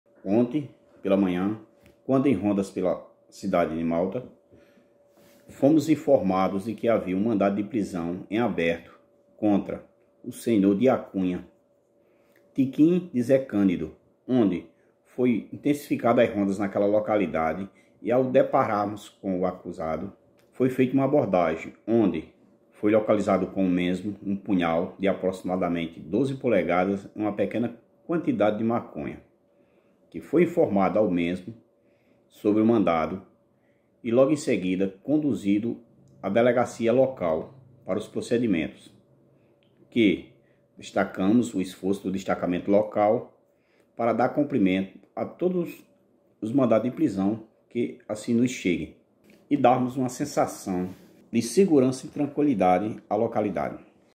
Acusado de estupro é preso em Malta por PM's da 2º CIA
Contra o homem havia mandado de prisão em aberto pelo crime